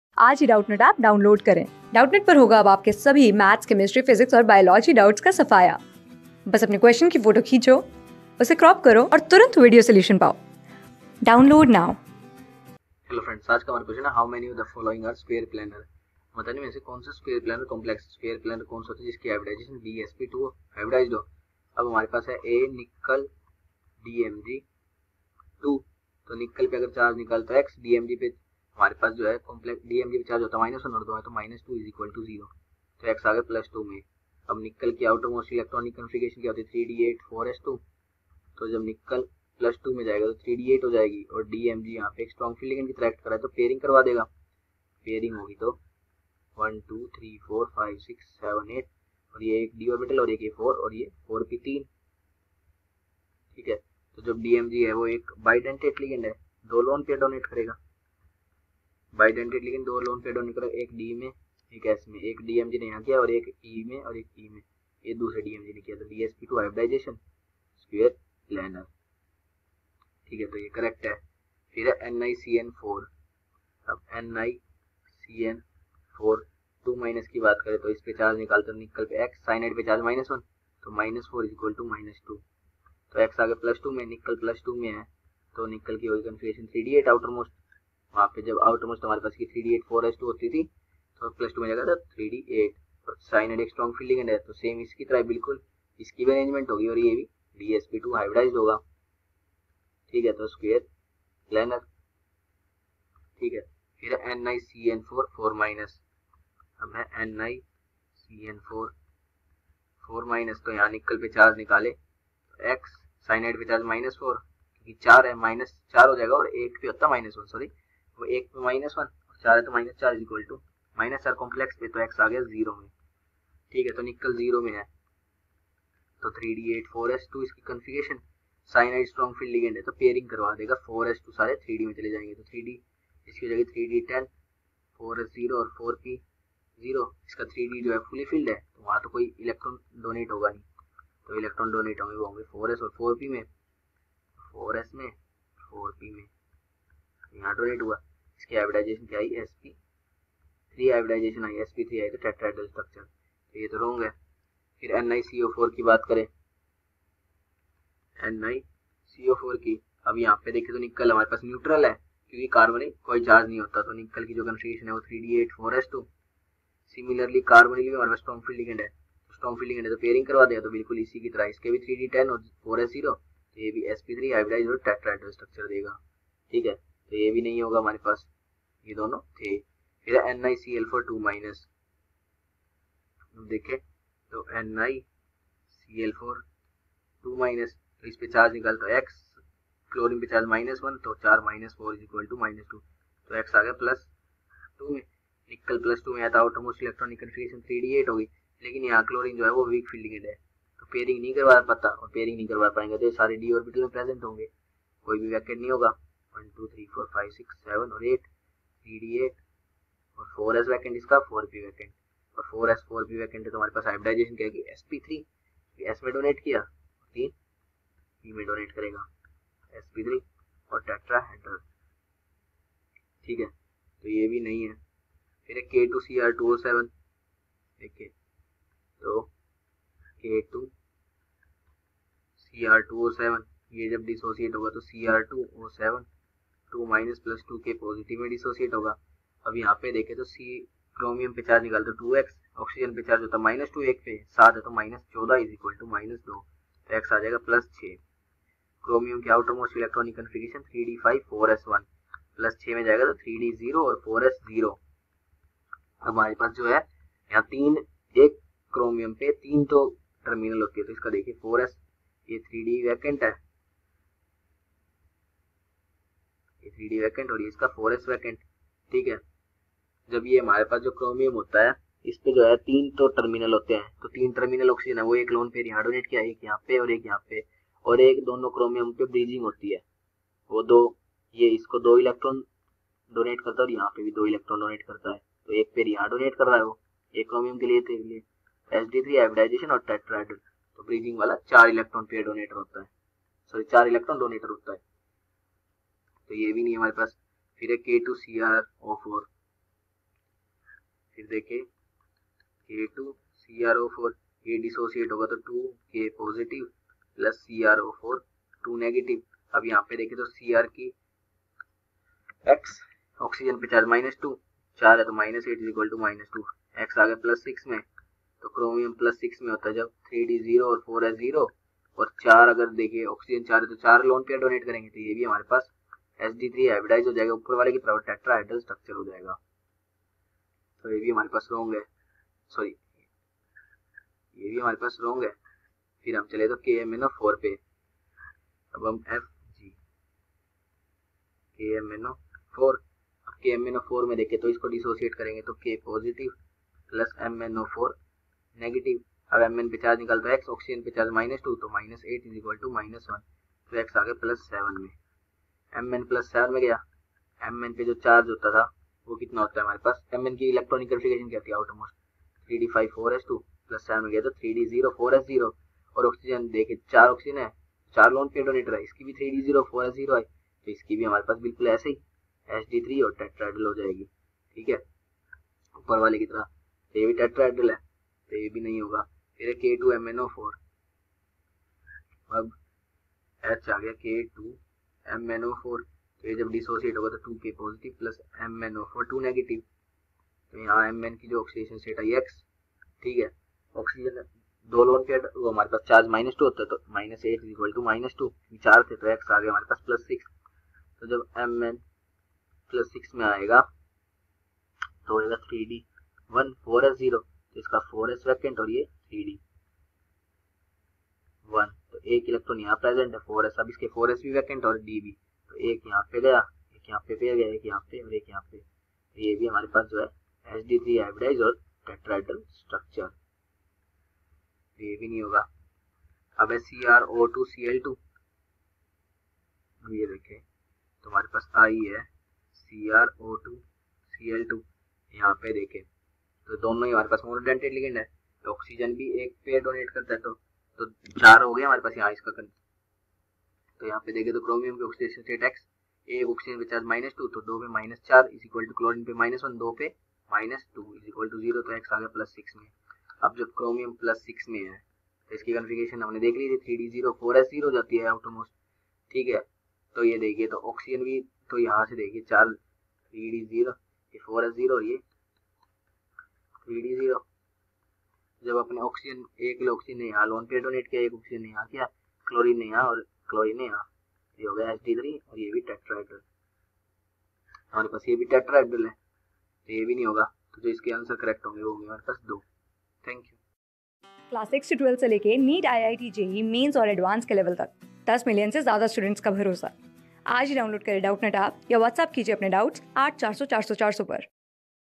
How many of the following are square palanar? (a) Ni(dmg)_(2) (b) [Ni(CN)_(4)]^(2-) (c ) [Ni(CN)...
How many of the following are square palanar? (a) Ni(dmg)_(2) (b) [Ni(CN)_(4)]^(2-) (c ) [Ni(CN)_(4)]^(4-) (d) [Ni(CO)_(4)] (e ) [Ni(Cl)_(4)]^(2-) (f) K_(2)Cr_(2)O_(7) (g) K_(2)CrO_(4) (h) KMnO_(4) (i) K_(2)MnO_(4) (j) CrO_(2)Cl_(2)
Class: 12
Subject: CHEMISTRY
Chapter: COORDINATION COMPOUNDS
Board:IIT JEE

👉 Have Any Query? Ask Us.